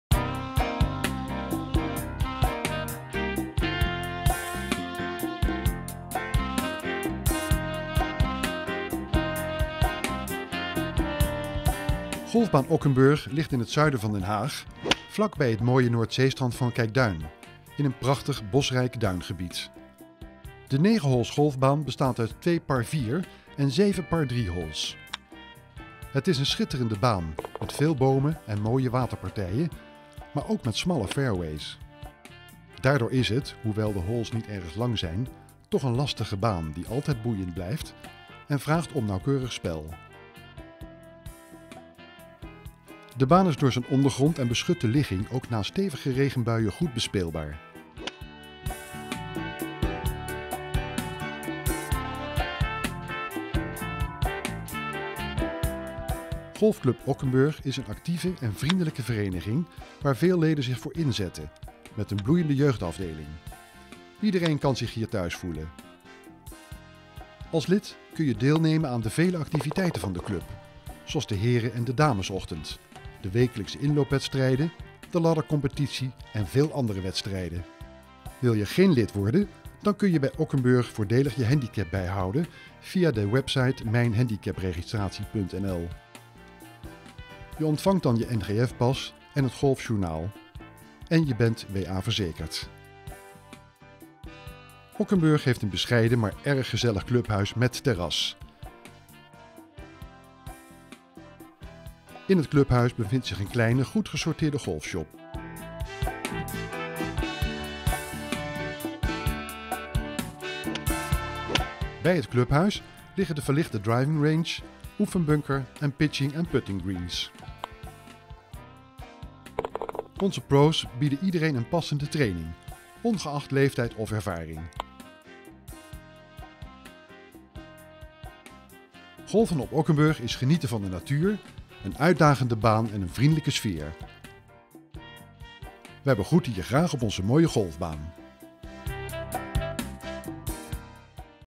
Golflessen op Golfbaan Ockenburgh
Volg nu zo vaak golflessen als je wilt op Golfbaan ockenburg. Voor een vast bedrag per seizoen, via Birdy Lessons.